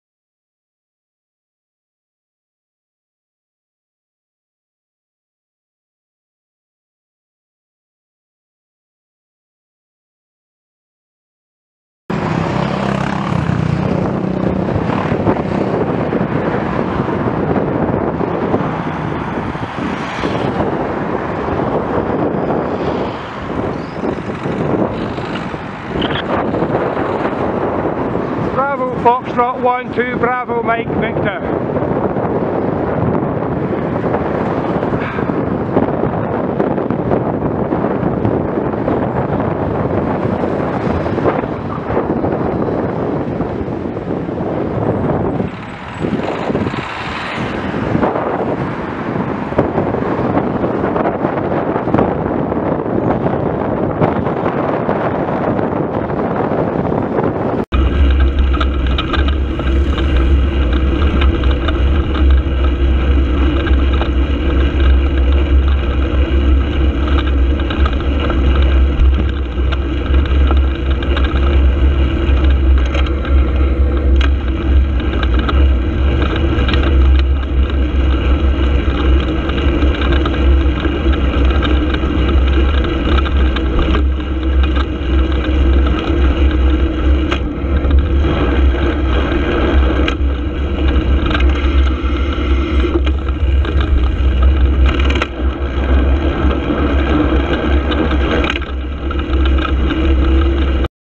BF12BMV overtake on corner forces oncoming traffic into bus lane
Location: Stoney Stanton Road, corner beside police station between St Pauls Road and Awson Street.

Friday 26th May 2017 18:05